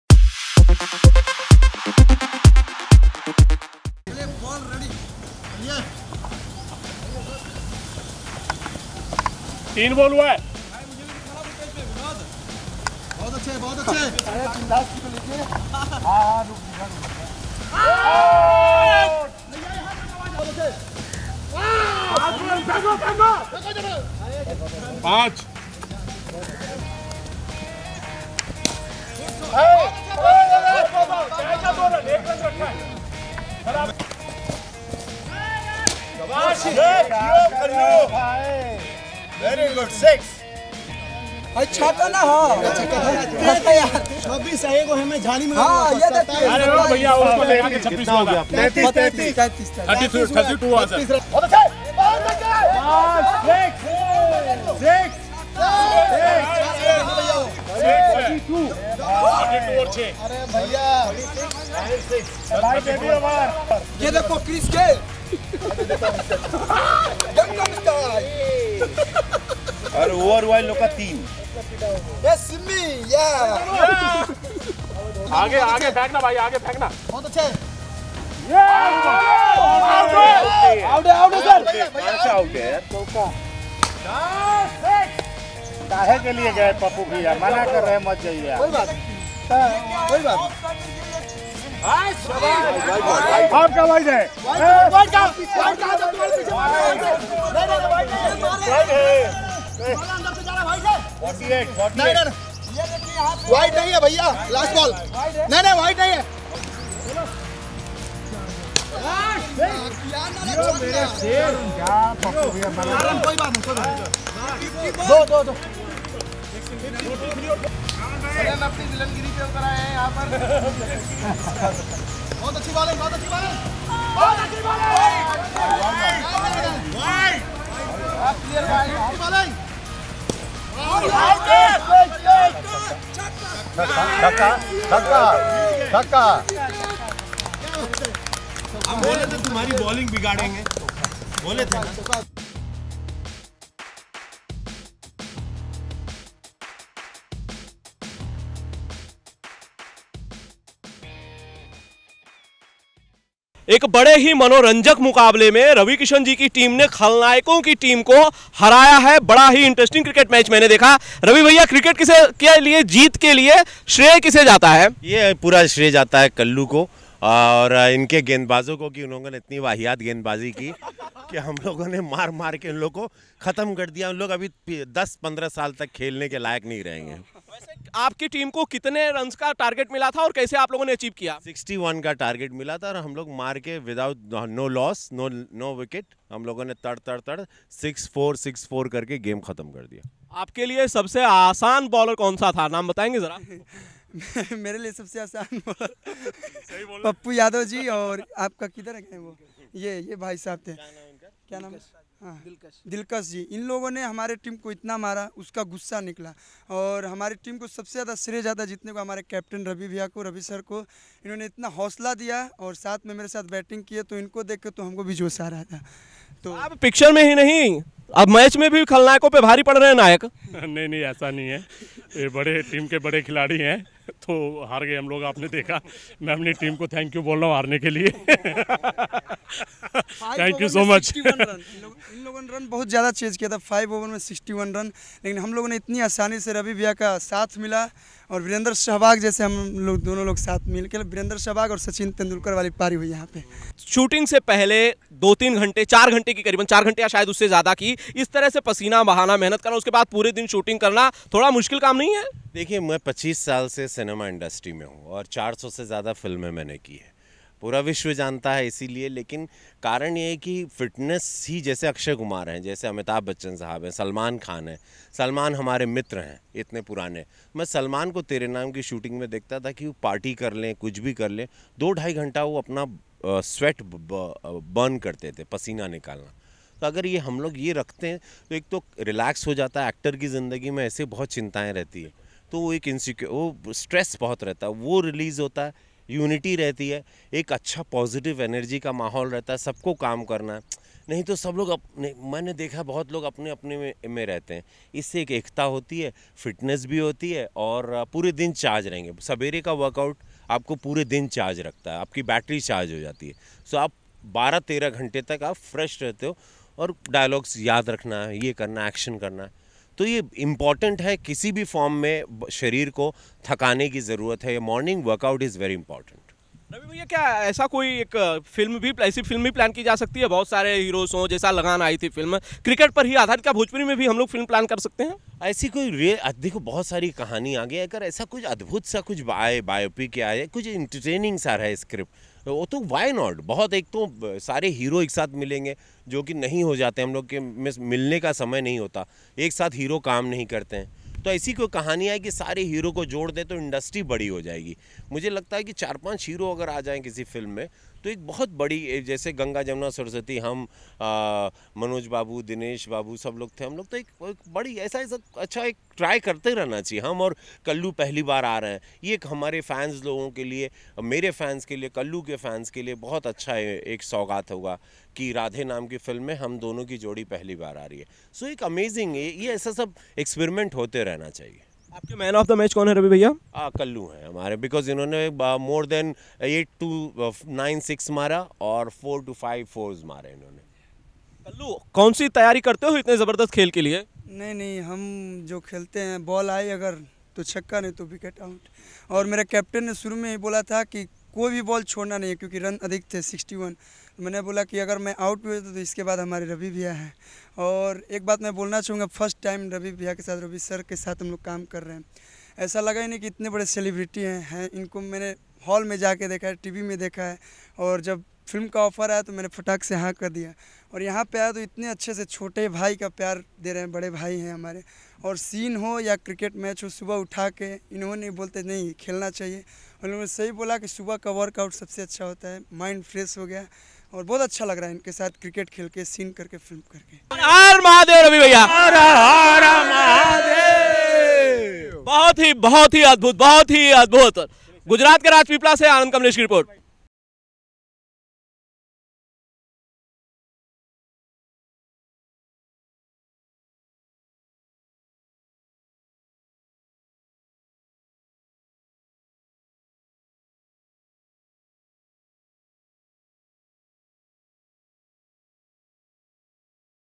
कल्लू और रवि किशन का क्रिकेट मैच देखिये | Ravi Kishan VS Kallu | Bhojpuri Cricket Match
Bhojpuri Megastar Ravi Kishan plays Cricket match with singer turned actor Kallu Ji. The Pair wiill be seen in Neha Shree's next movie ' Radhe '. Enjoy the video.